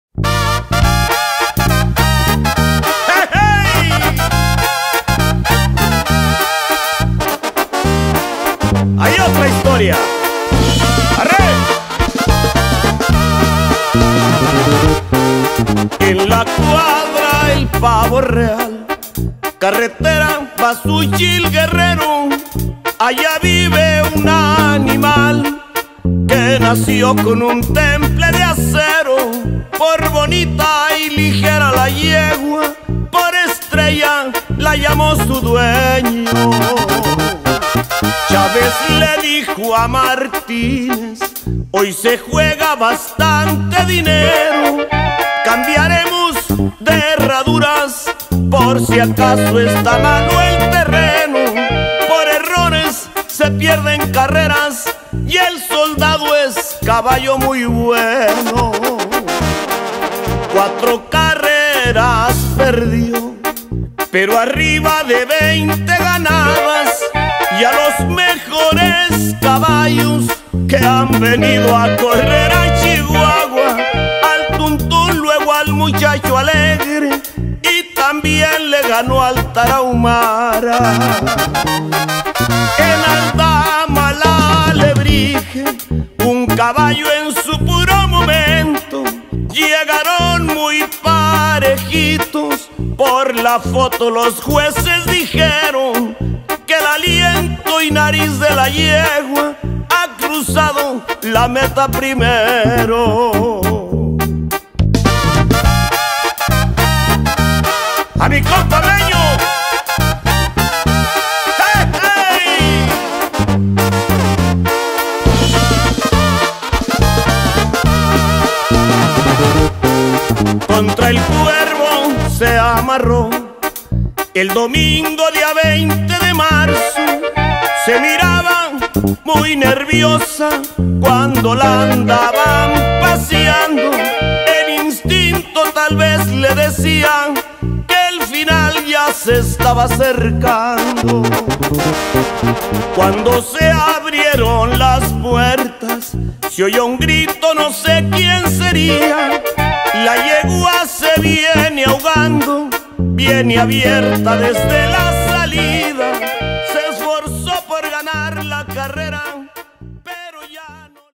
El Carbonero VS El JR en 250yds
Carril El Manantial
El Carbonero C. Flaco/Sifuentes
VS
El JR
C. F/S Racing
en 250yds

Drone: El Gallo Ponce
No tenemos los derechos de esta musica.